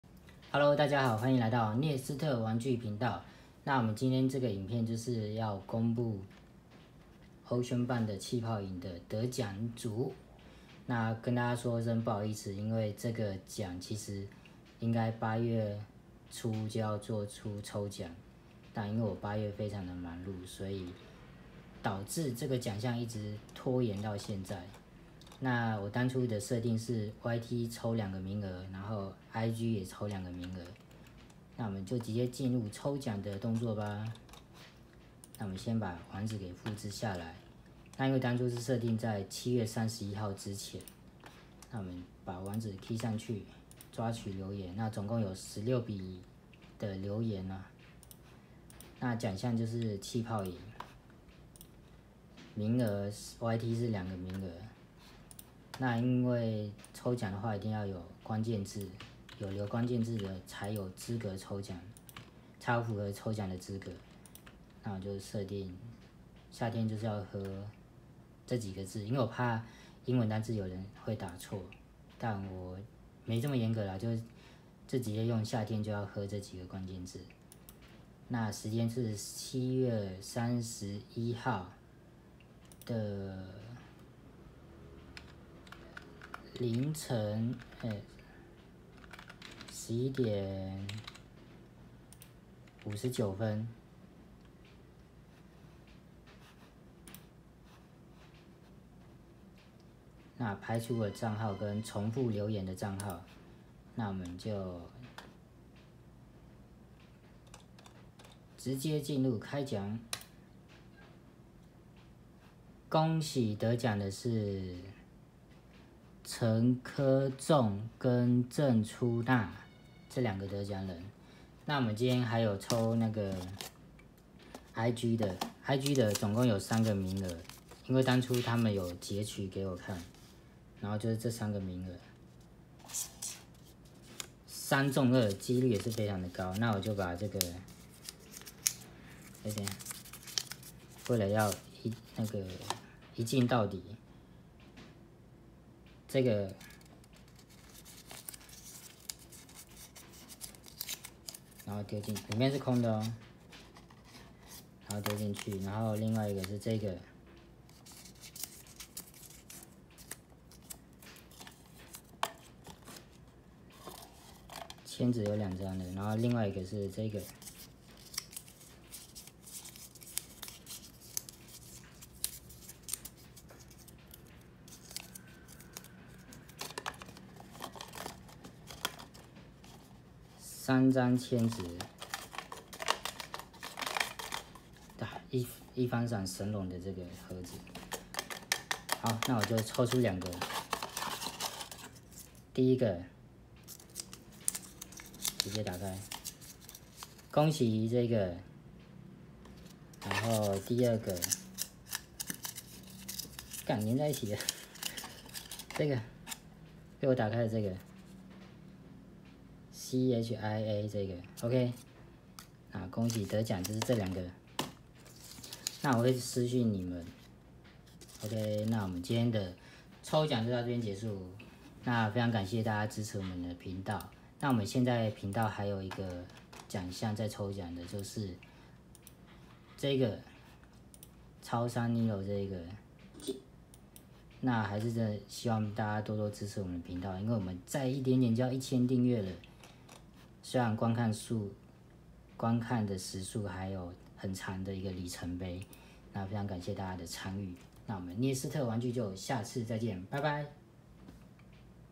【涅斯特玩具抽獎影片】OCEANBOMB氣泡飲得獎名單出爐瞜！感謝大家的參與！！
非常抱歉這個抽獎拖延的有點久，
因八月非常忙碌，還望大家見諒!!

恭喜得獎者~我會私訊您們哦!

涅斯特玩具頻道感謝您們長期的支持與參與~

那我們就下部開箱影片見!